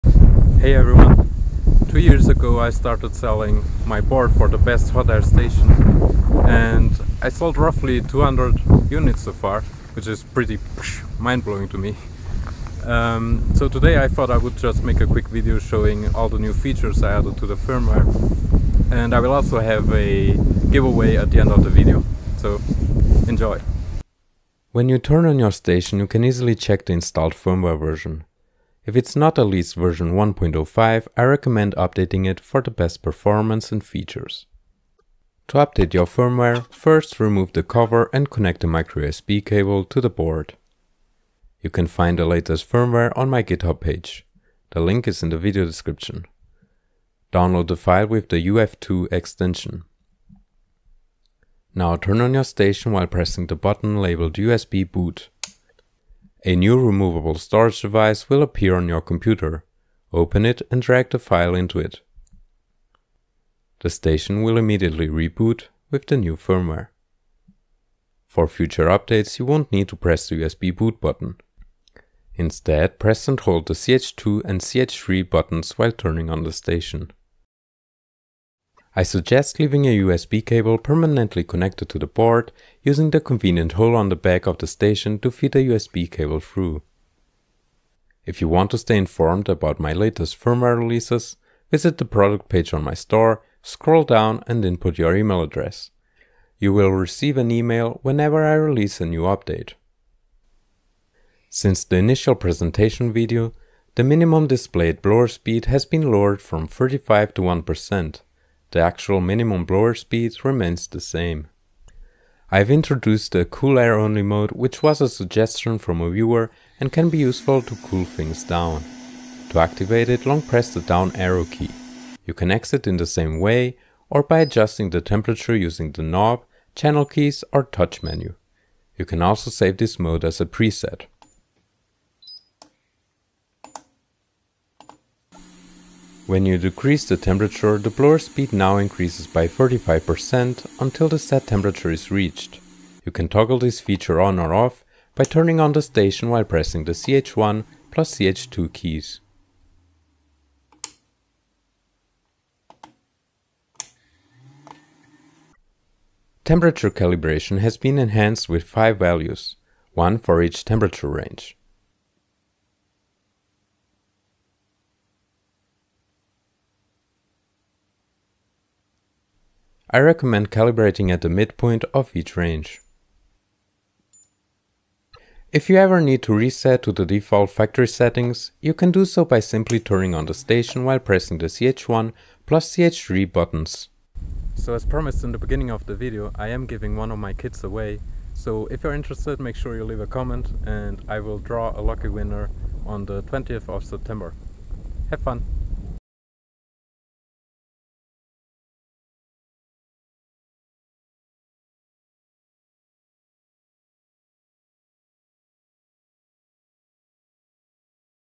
[GIVEAWAY] New features for the best BST-863 improvement board!
There are a few other changes I made to improve safety and temperature accuracy, you can read the complete changelog and download the firmware on github

If you want to participate in the giveaway leave a comment under this video (youtube only) and make sure you make it clear you are participating. I will draw a random comment on the 20th of september and upload a video then, the winner will get a free upgrade kit with free shipping (I will not be responsible for any customs duty taxes your country might charge).

45º Nozzles
*Banggood
Station EU/USA/CN

Chapters:
0:00 Intro
0:24 Check firmware version
0:35 Update firmware
1:40 New features
3:04 Outro